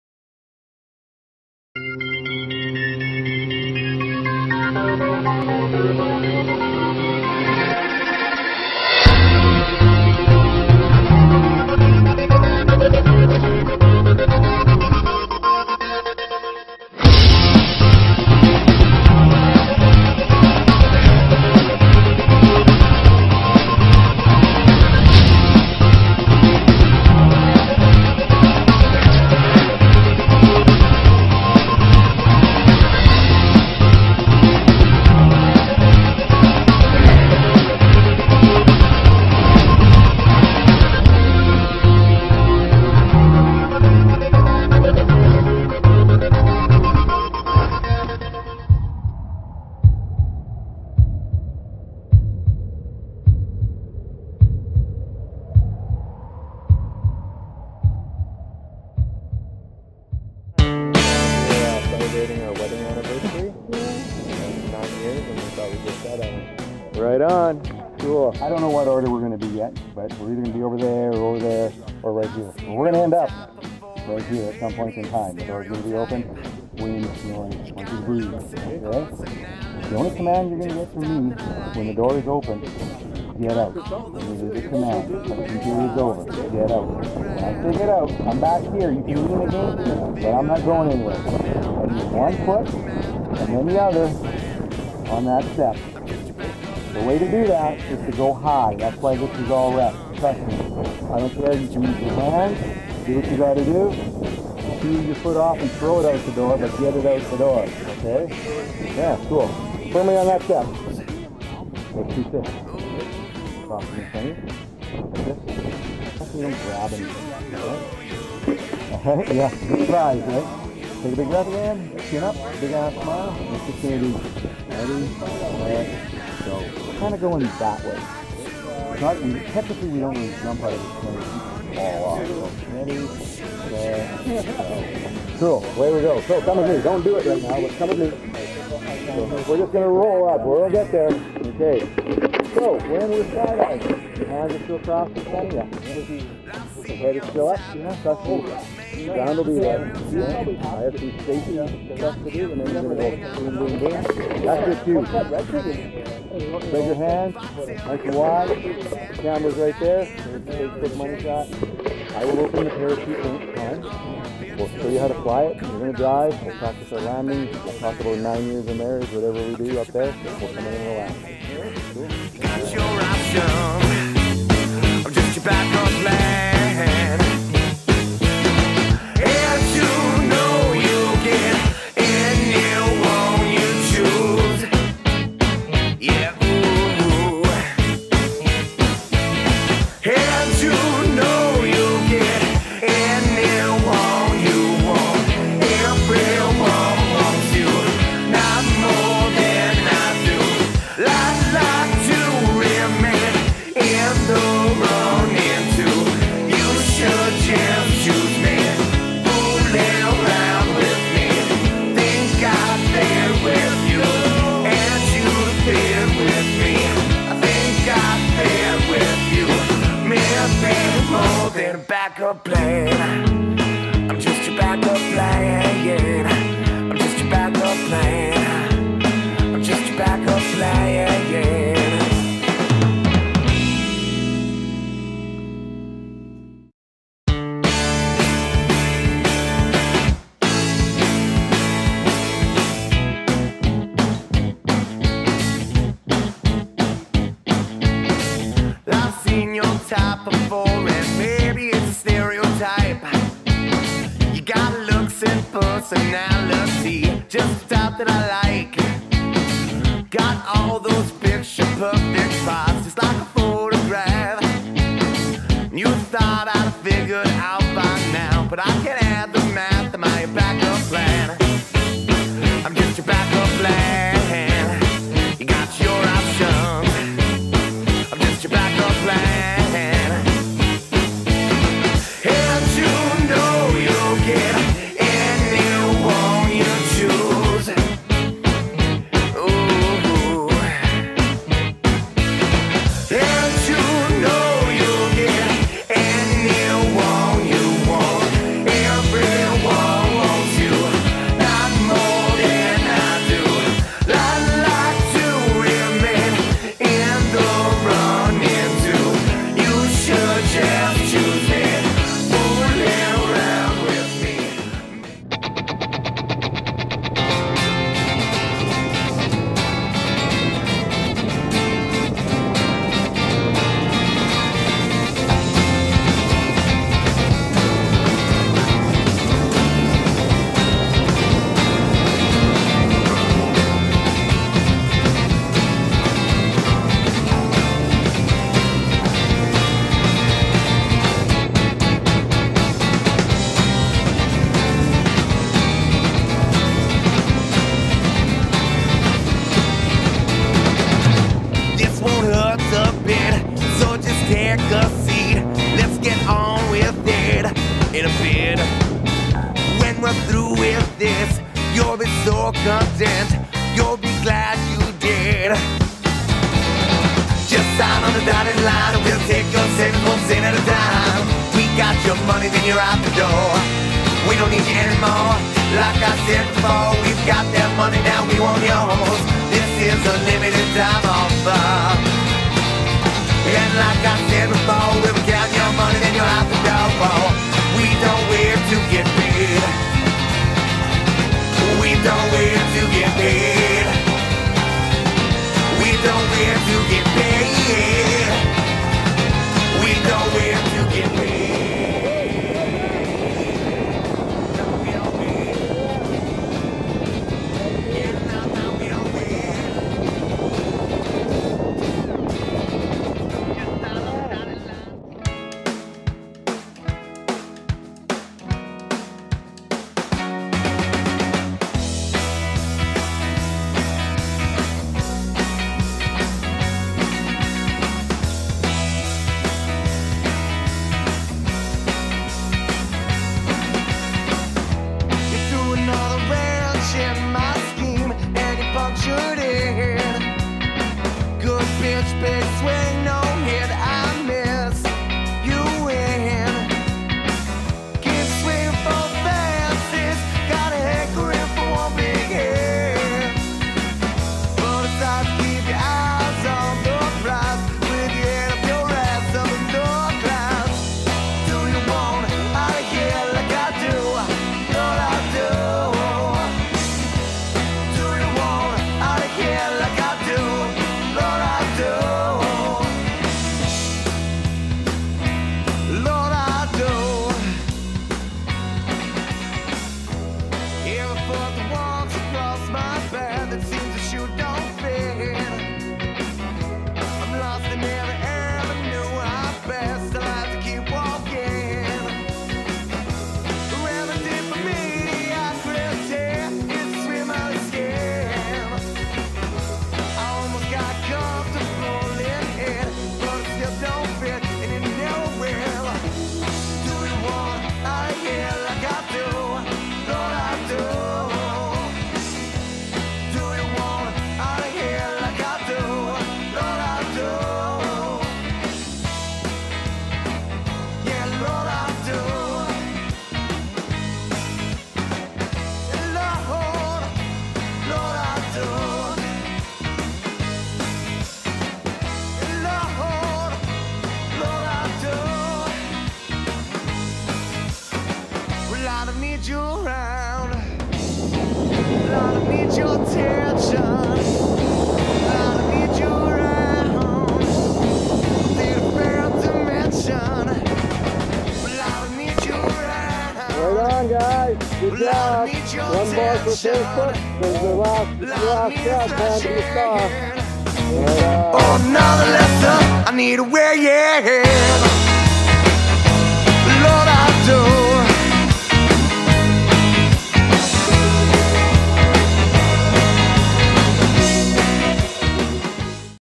Skydive South Sask Tandem Skydive - Jeremy Kerr
A Tandem Jump With Skydive South Sask - Jeremy Kerr